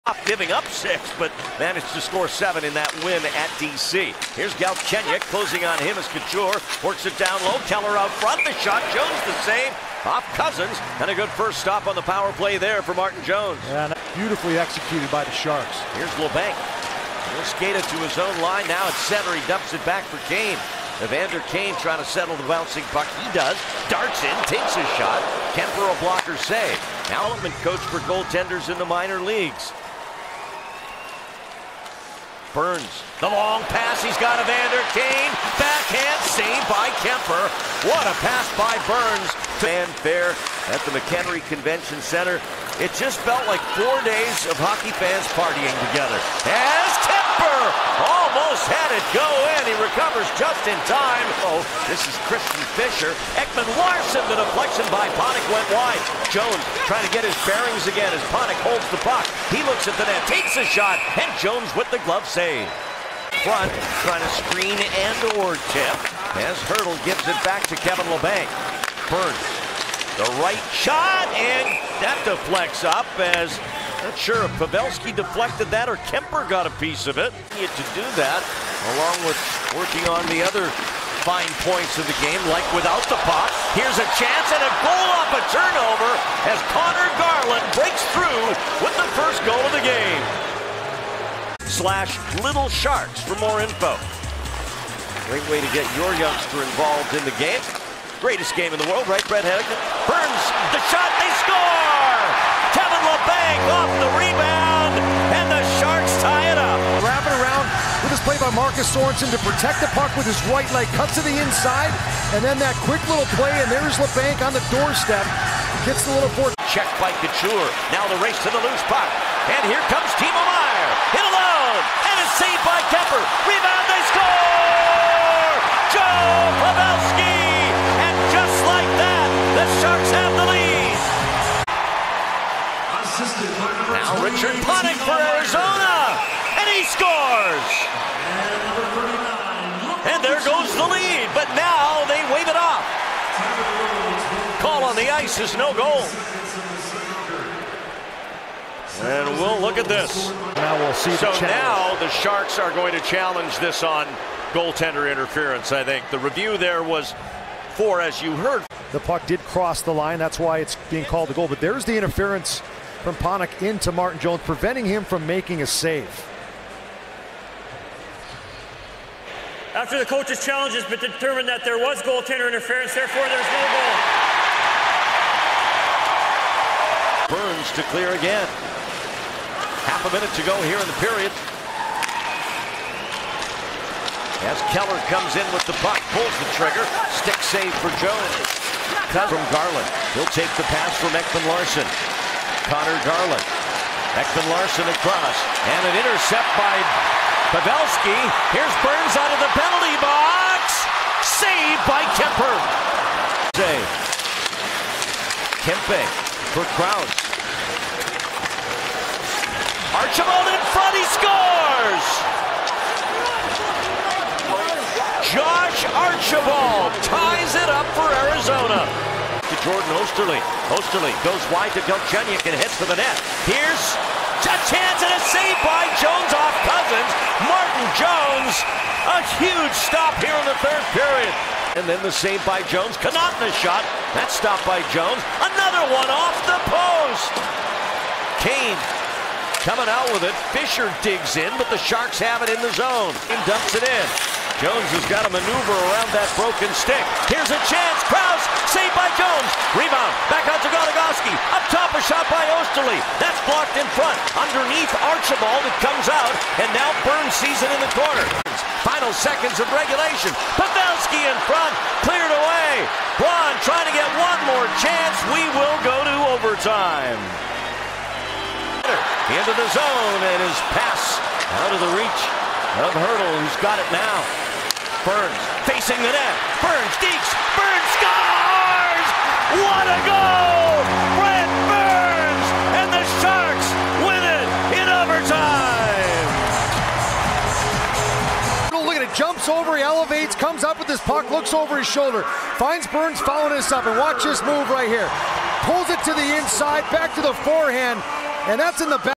NHL Highlights | Coyotes vs. Sharks - Feb. 2, 2019
Brent Burns scored a stunning goal in overtime to lift the Sharks to a 3-2 win over the Coyotes Saturday.